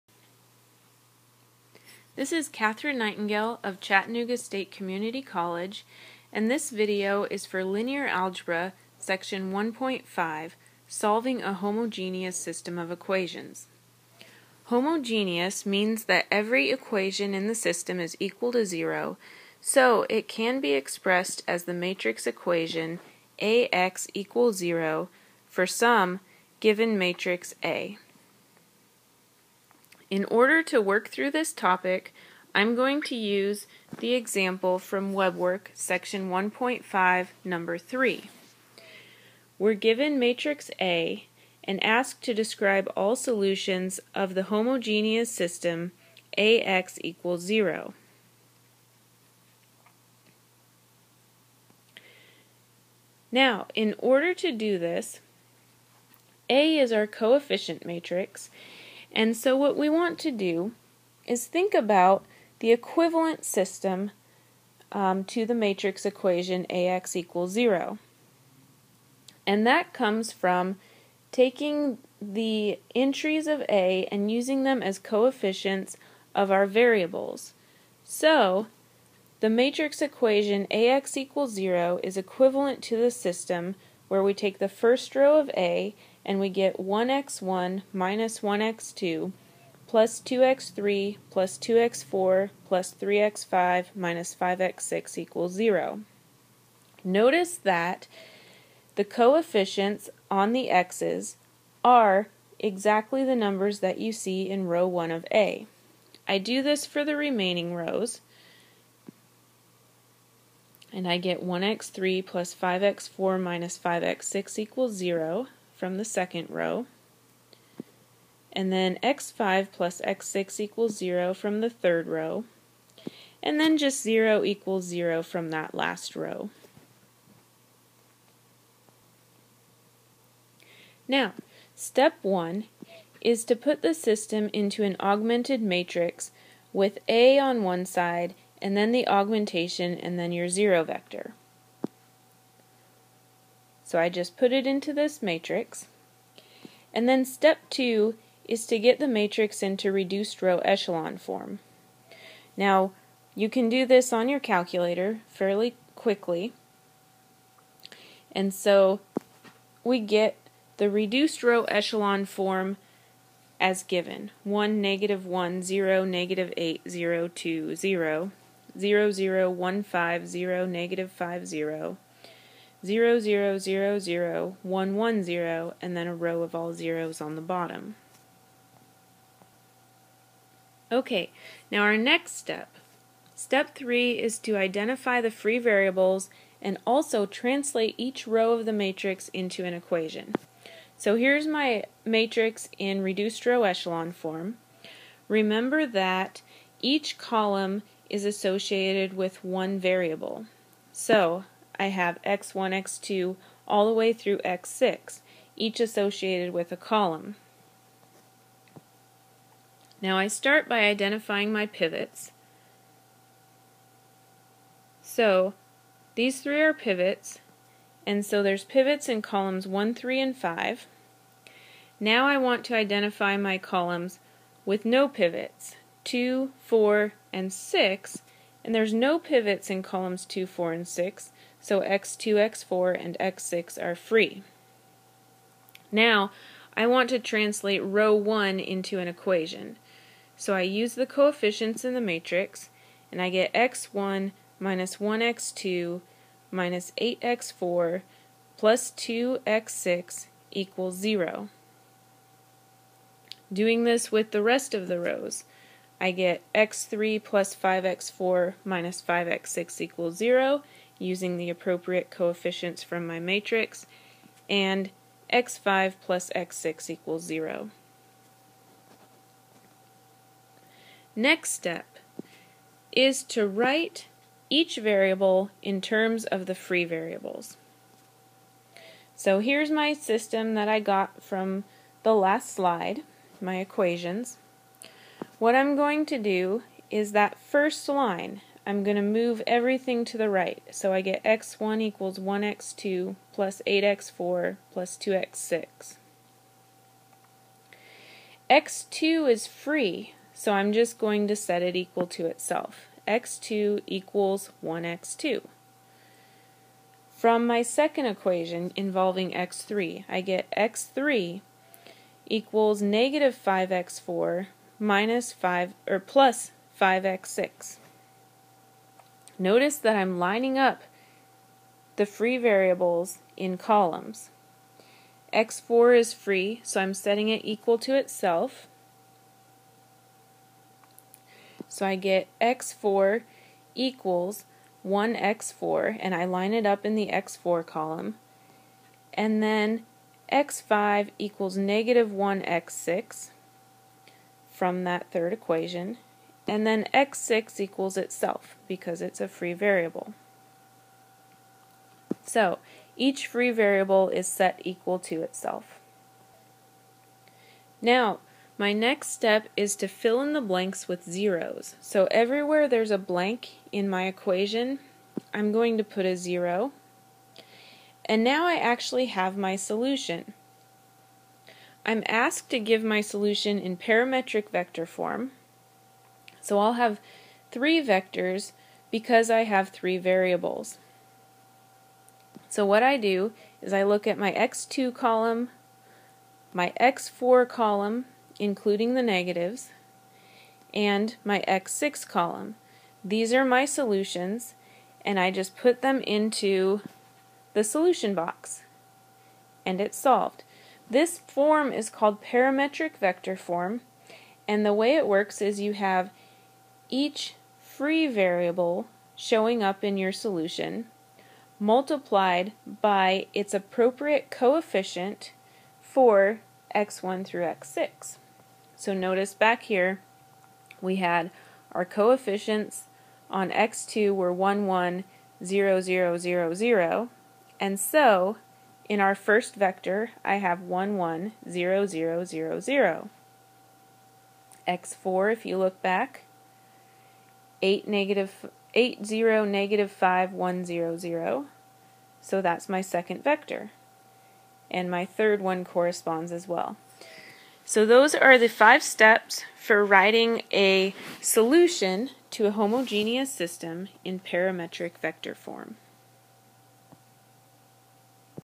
Linear Algebra Section 1.5 Solving a Homogeneous System
This video shows the steps for solving a homogeneous system of equations and writing the solution in Parametric Vector Form. The example used is from WebWork Section 1.5 Problem 3.